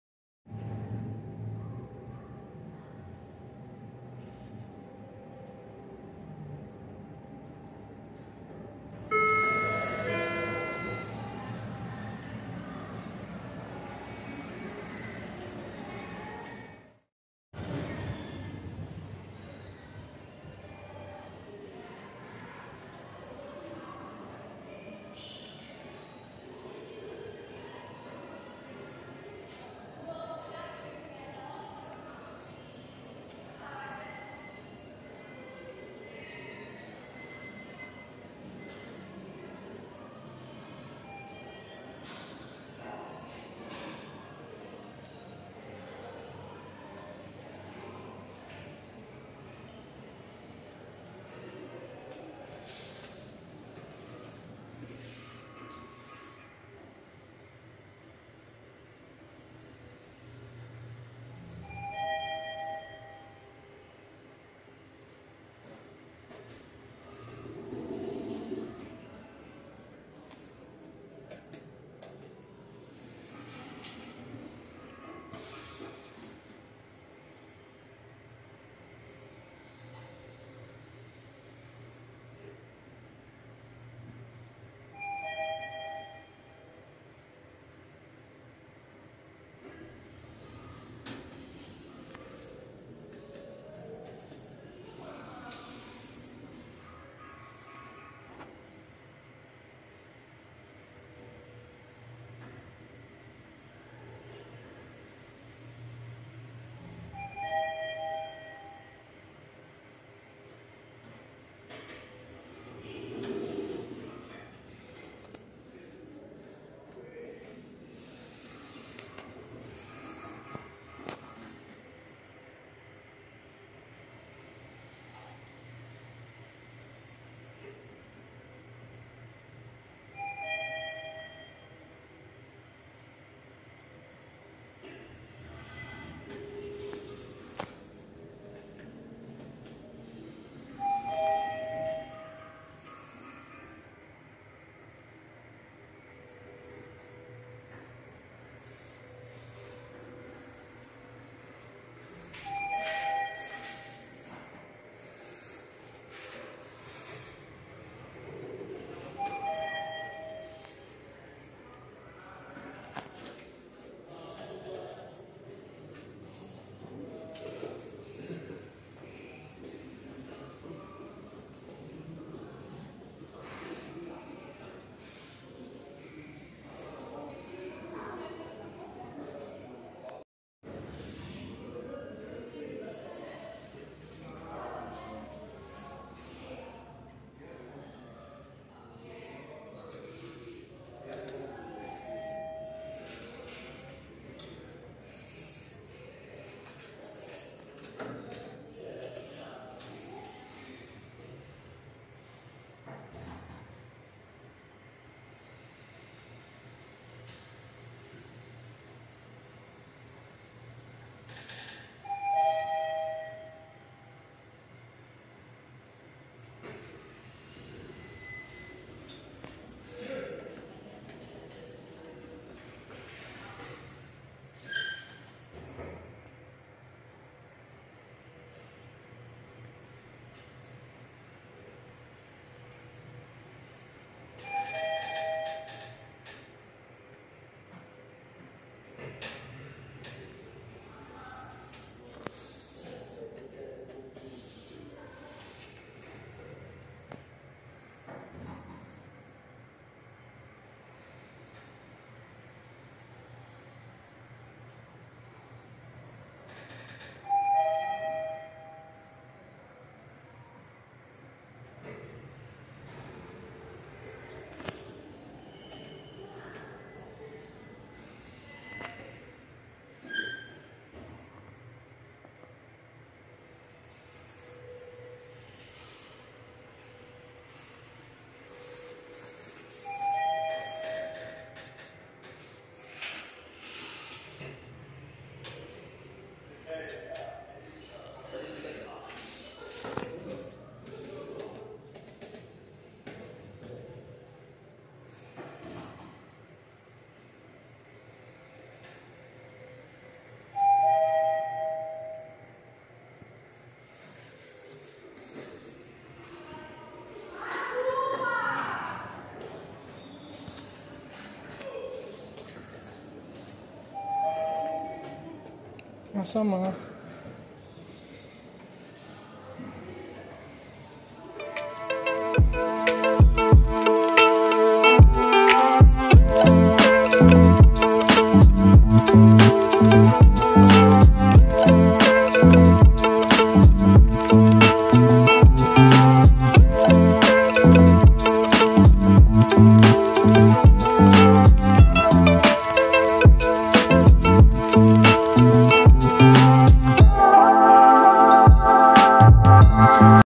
Brand New Mitsubishi NEXIEZ-MRL Passenger Lifts - Bumi Pakubuwono, Jakarta
A new Mitsubishi 3rd Gen in a new strip mall at Pakubuwono - Bumi Street, wonder why does these units were using a single HOP and separate calls (even though both units has a same company and model)

[Video Information]
• Shooting Date: December 22nd 2023
• Camera: Vivo S1 Pro
• Edited with: Filmora 9

[Building Information]
• Building Name: Bumi Pakubuwono
• Building Type: Shopping Mall
• Location: Pakubuwono (Bumi Rd.)

[Lift / Elevator Information]
• Brand: Mitsubishi
• Model / Fixtures: NEXIEZ-MRL Traction / Mitsubishi 3rd Gen
• Installer: PT Mitsubishi Jaya
• Installation Year: 2023
• Type: Passenger Lift
• Capacity: 14 Persons / 1050 Kg
• Serving: GF, L1, L2 (3 Floors)
• Non-Serve Floors: None
• History: Original

-Like my Facebook Page: VT Elevators (@vtelevators93)

Note: You CANNOT Reuse or Reupload this content without the Admin's permission
Always Remember: Lift/Elevator Filming it's NOT A CRIME!